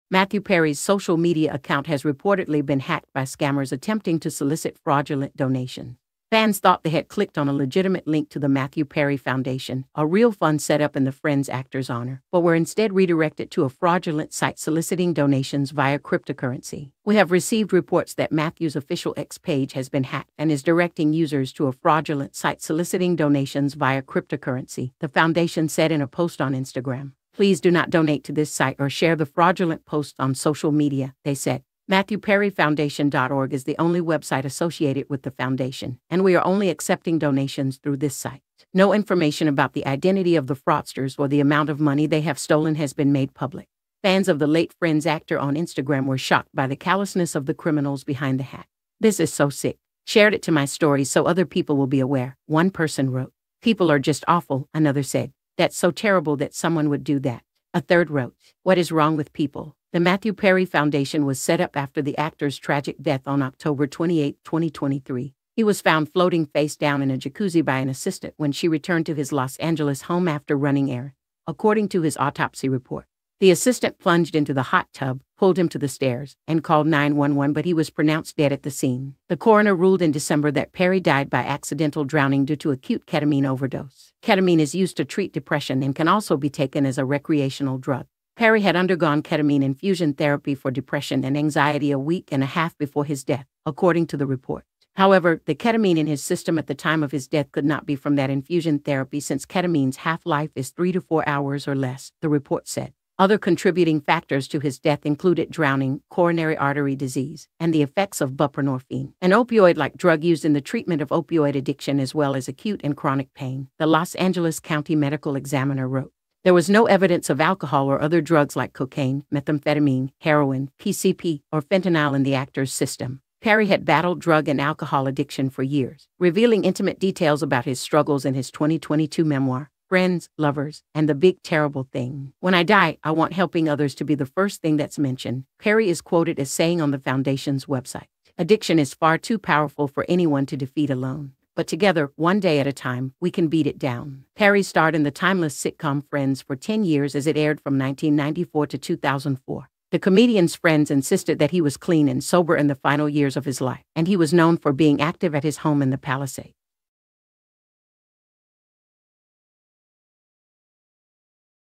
Matthew Perry targeted by sick hackers as fans warned of ‘fraudulent site demanding crypto cash
Matthew Perry targeted by sick hackers as fans warned of ‘fraudulent site demanding crypto cash in star’s memory’

MATTHEW Perry's social media account has reportedly been hacked by scammers attempting to solicit fraudulent donations.

Fans thought they had clicked on a legitimate link to the Matthew Perry Foundation — a real fund set up in the Friends actor's honor — but were instead redirected to a fraudulent site soliciting donations via cryptocurrency.

“We have received reports that Matthew’s official X page has been hacked and is directing users to a fraudulent site soliciting donations via cryptocurrency," the foundation said in a post on Instagram.

"Please do not donate to this site or share the fraudulent posts on social media," they said.

"MatthewPerryFoundation.org is the only website associated with the Foundation, and we are only accepting donations through this site.”

No information about the identity of the fraudsters or the amount of money they have stolen has been made public.

Fans of the late Friends actor on Instagram were shocked by the callousness of the criminals behind the hack.

"This is so sick!! Shared it to my story so other people will be aware," one person wrote.

"People are just awful," another said.

"That's so terrible that someone would do that!" a third wrote. "What is wrong with people?"

The Matthew Perry Foundation was set up after the actor's tragic death on October 28, 2023.

He was found floating face down in a jacuzzi by an assistant when she returned to his Los Angeles home after running errands, according to his autopsy report.

The assistant plunged into the hot tub, pulled him to the stairs, and called 911 but he was pronounced dead at the scene.

The coroner ruled in December that Perry died by accidental drowning due to acute ketamine overdose.

Ketamine is used to treat depression and can also be taken as a recreational drug.

Perry had undergone ketamine infusion therapy for depression and anxiety a week-and-a-half before his death, according to the report.

However, the ketamine in his system at the time of his death "could not be from that infusion therapy since ketamine's half-life is 3 to 4 hours or less," the report said.

Other contributing factors to his death included "drowning, coronary artery disease, and the effects of buprenorphine," an "opioid-like drug used in the treatment of opioid addiction as well as acute and chronic pain," the Los Angeles County Medical Examiner wrote.

There was no evidence of alcohol or other drugs like cocaine, methamphetamine, heroin, PCP, or fentanyl in the actor's system.

Perry had battled drug and alcohol addiction for years, revealing intimate details about his struggles in his 2022 memoir, Friends, Lovers, and the Big Terrible Thing.

"When I die, I want helping others to be the first thing that's mentioned," Perry is quoted as saying on the foundation's website.

"Addiction is far too powerful for anyone to defeat alone. But together, one day at a time, we can beat it down."

Perry starred in the timeless sitcom Friends for 10 years as it aired from 1994 to 2004.

The comedian's friends insisted that he was clean and sober in the final years of his life and he was known for being active at his home in the Palisades.

Perry had a brief stint on life support in 2018 and was in a coma for two weeks after several medical episodes due to his past drug use.

He endured more than a half-dozen stomach surgeries and used a colostomy bag for nine months, in addition to suffering from pneumonia and an exploded colon.

After the news of Perry's death sank in, his co-stars, Jennifer Aniston, Courteney Cox, David Schwimmer, Matt LeBlanc, and Lisa Kudrow, shared a joint statement.

"We are all so utterly devastated by the loss of Matthew. We were more than just cast mates. We are a family," the statement reads, first obtained by People.

"There is so much to say, but right now we’re going to take a moment to grieve and process this unfathomable loss."